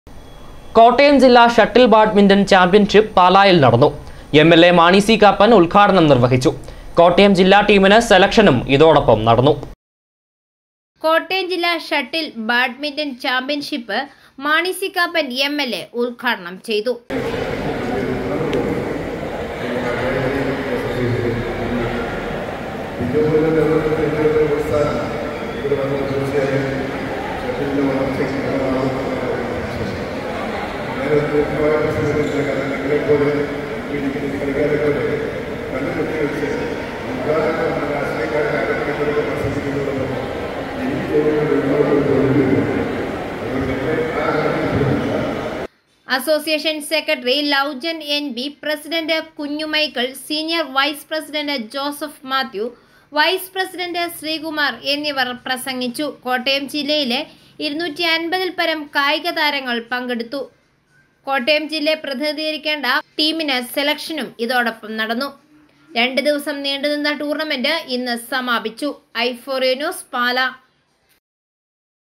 ജില്ല ഷട്ടില്‍ ബാഡ്മിന്‍ന്റ്ണ്‍ ചാമ്പ്യന്‍ഷിപ്പ് പാലായില്‍ നടന്നു
കോട്ടയം ജില്ല ഷട്ടില്‍ ബാഡ്മിന്‍ന്റ്ണ്‍ ചാമ്പ്യന്‍ഷിപ്പ് പാലായില്‍ നടന്നു. എംഎല്‍എ  മാണി C കാപ്പന്‍  ഉല്‍ഘാടനം നിര്‍വഹിച്ചു.
കോട്ടയം ജില്ലാ ടീമിന് സെലക്ഷനും ഇതോടൊപ്പം നടന്ന